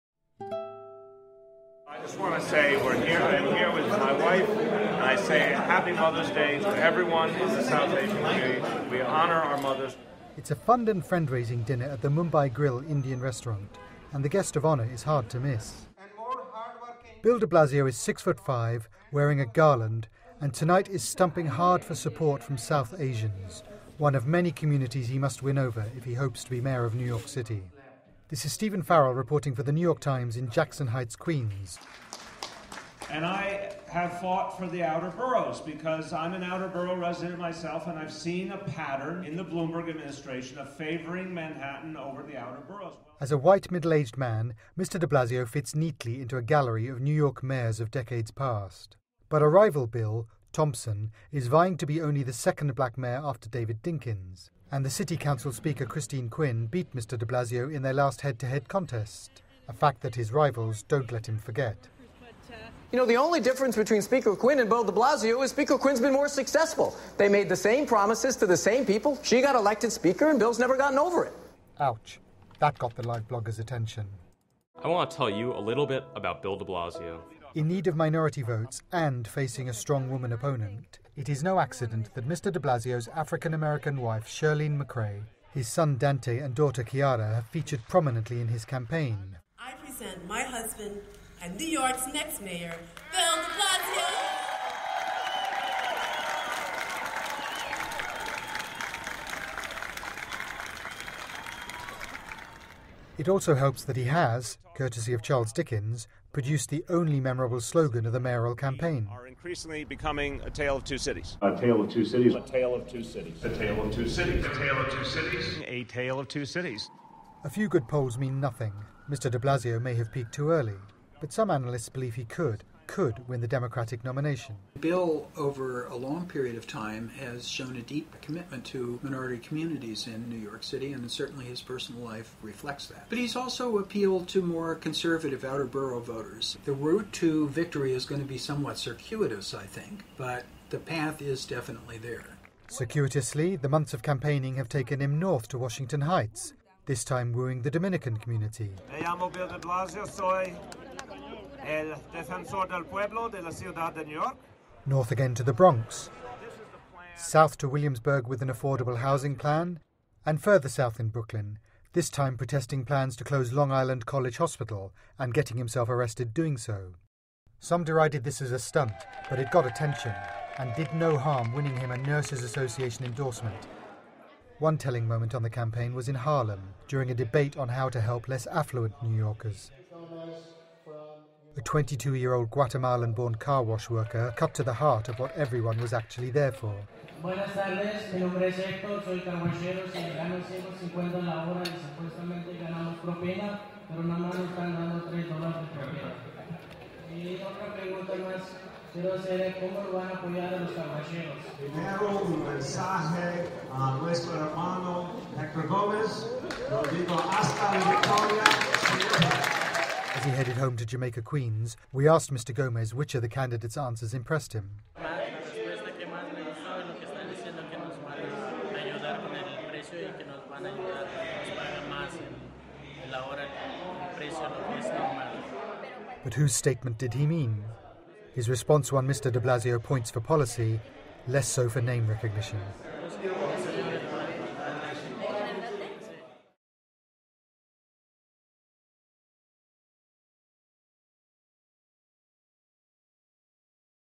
Bill de Blasio: "The Tall, White One"
On the mayoral election trail, New York City's public advocate, Bill de Blasio, has adopted the campaign slogan "A Tale of Two Cities" in his bid to win the Democratic nomination.